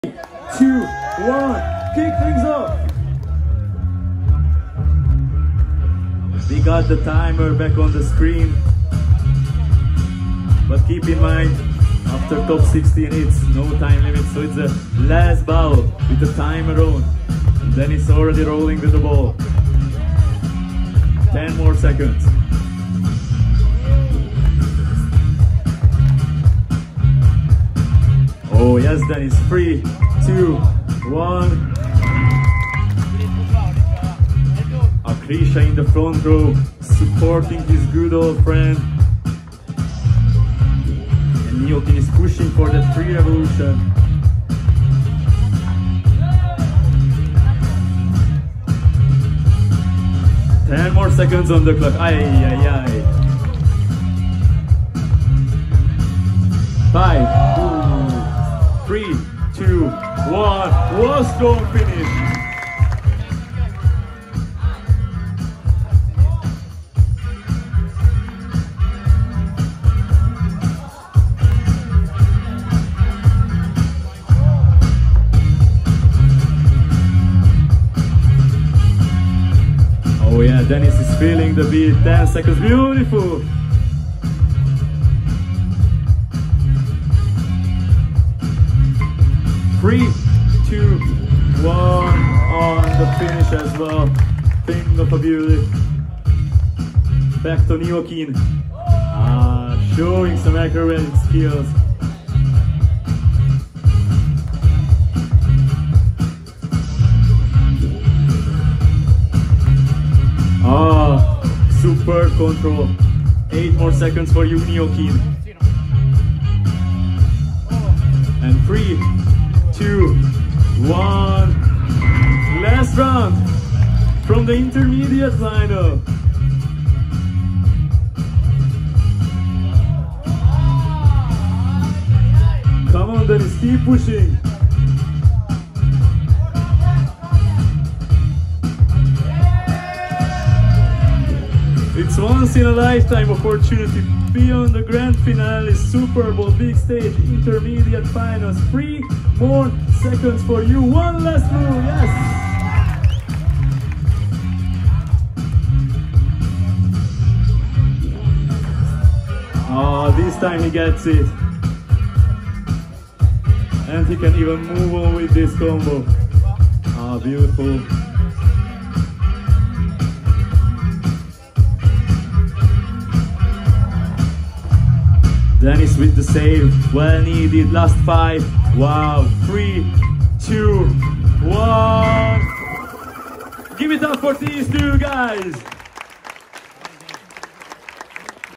Niokin vs Di-sS - Intermediate Final | Super Ball 2023
Niokin (MYS) vs Di-sS (UKR) - Intermediate Final | Super Ball World Open Freestyle Football Championships 2023 (Prague)

Winner: Di-sS

Partners: @superballlive
Freestylers: @anashakimianas, @Di-sS
Thumbnail: @lotaredits